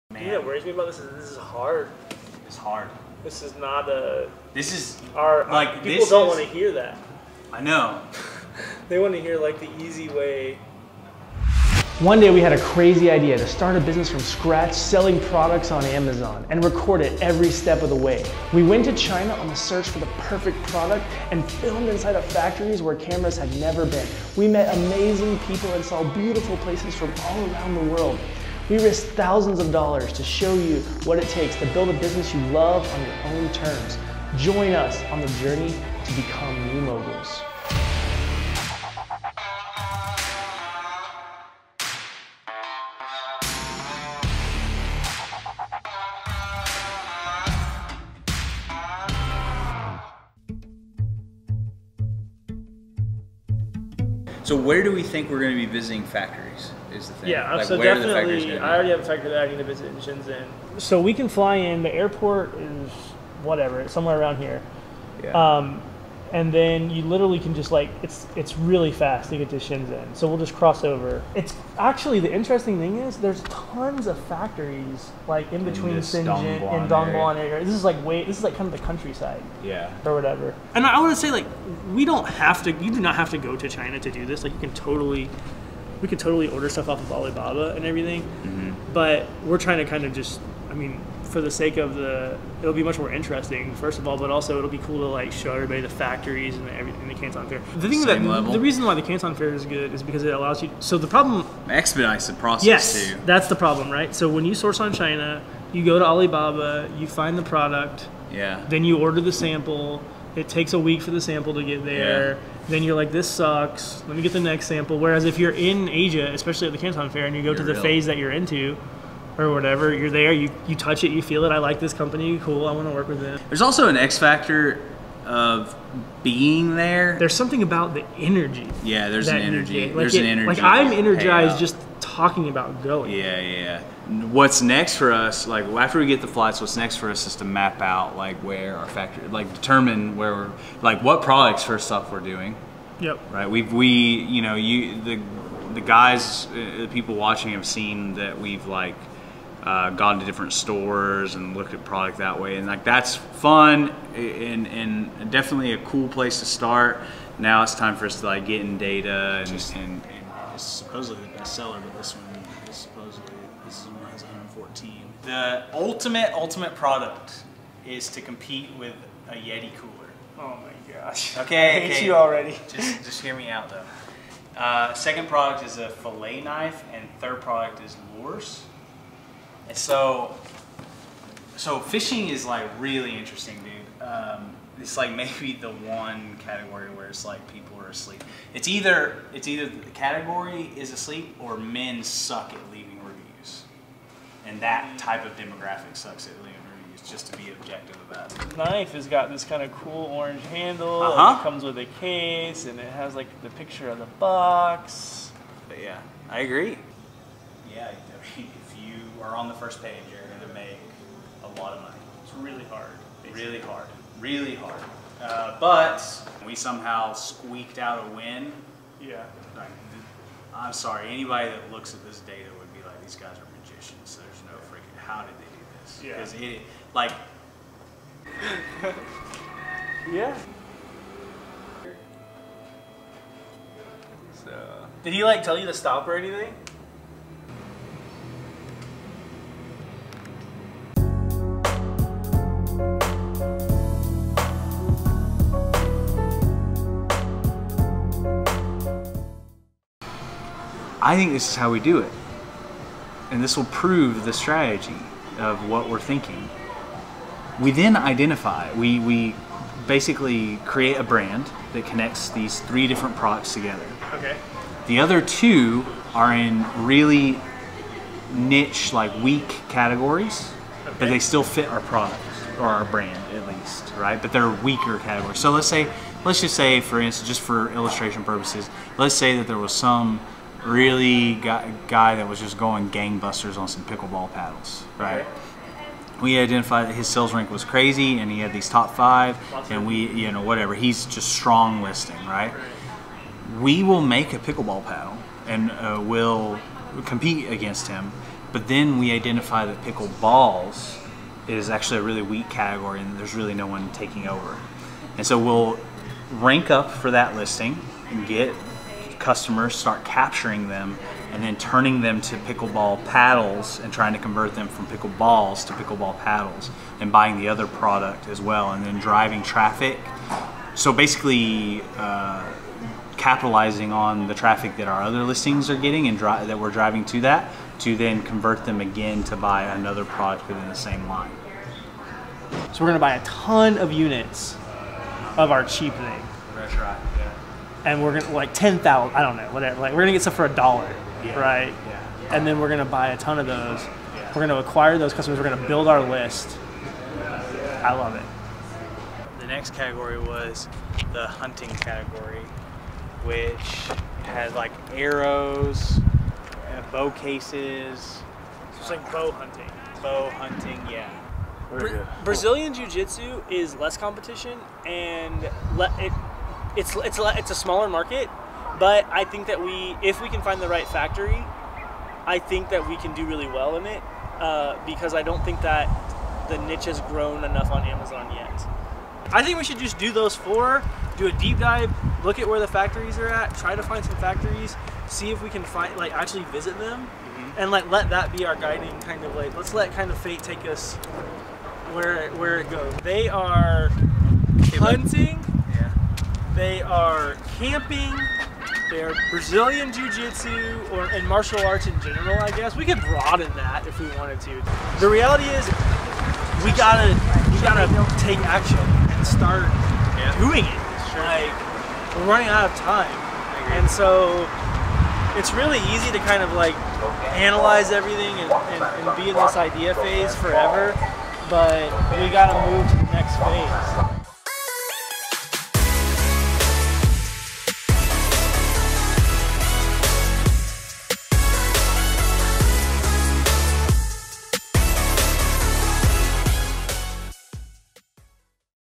Discovering Our New Amazon FBA Formula: Starting FBA From Scratch. New Mogul S1E2
In this episode, we deep dive into product research. A brand new idea is presented on building a "vertical". Follow the journey and learn to build an FBA business from scratch!